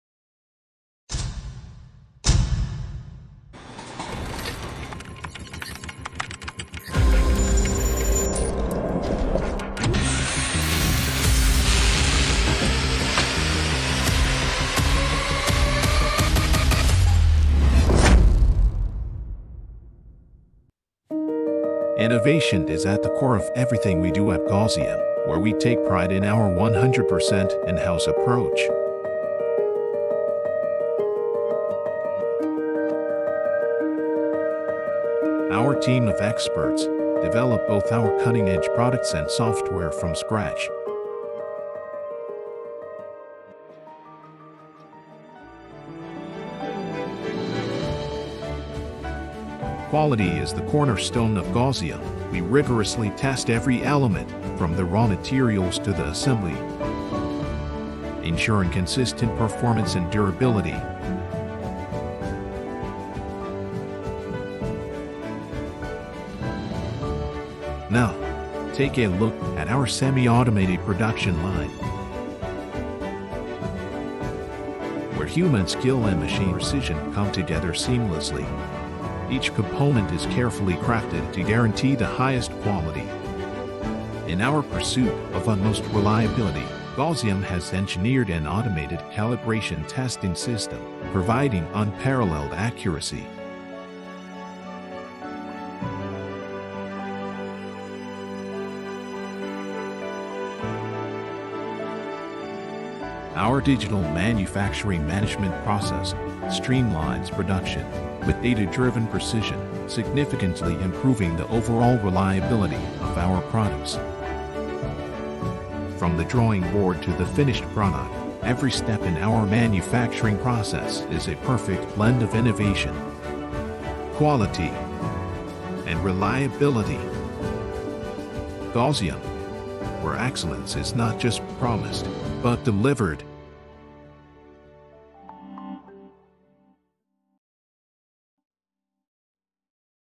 Behind the Scenes: The Birth of A Gausium Robot
Get ready for an exclusive peek behind the curtains at Gausium! Join us as we unveil the birth of our revolutionary robots in this behind-the-scenes video. From our in-house innovation hub to our production line, witness the seamless fusion of human expertise and cutting-edge technology. Don't miss out—experience the Gausium difference!